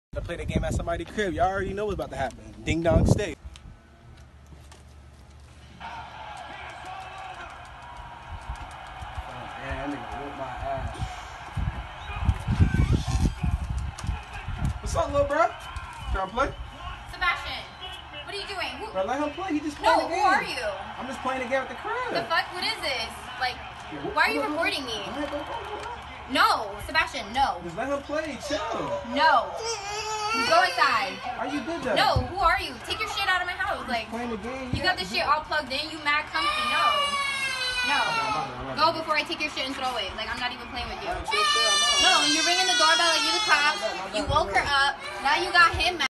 Guy Playing Video Games In The Front Yard Of A Random House
I was playing the game at a random person house & a little kid answers the door the parents didn’t let him play with me